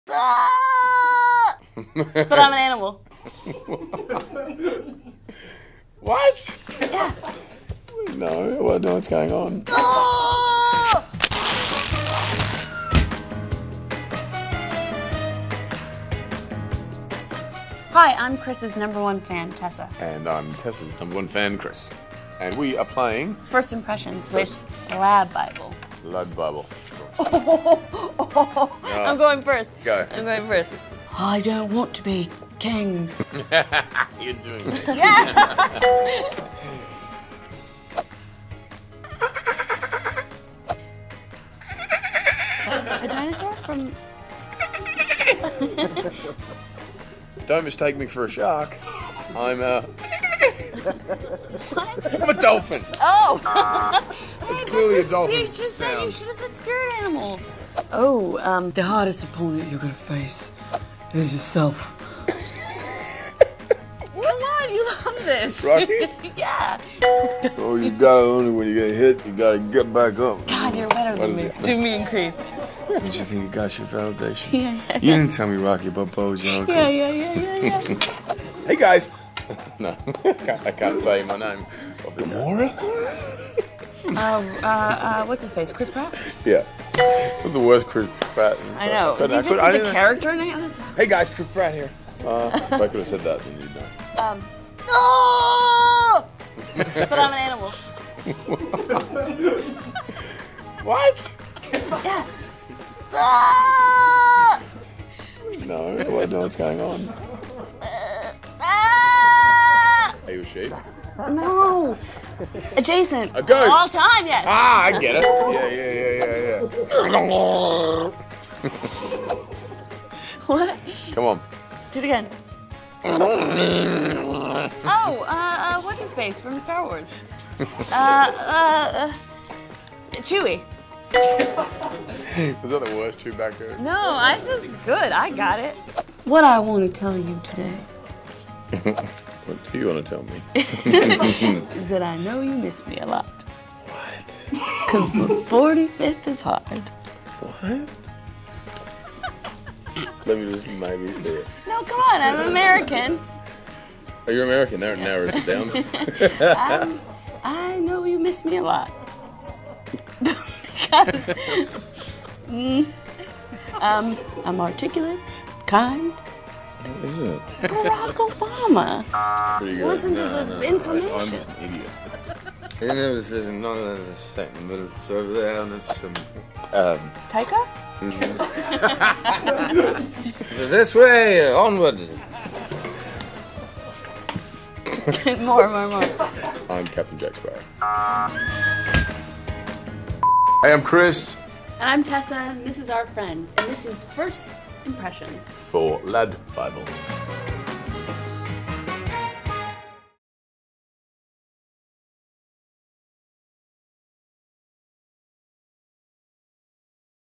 First Impressions | Chris Hemsworth's impression of Chris Pratt is hilarious!
The noises coming out of Chris Hemsworth and Tessa Thompson could definitely be described as alien 😂

Men in Black: International is released in cinemas on 14th June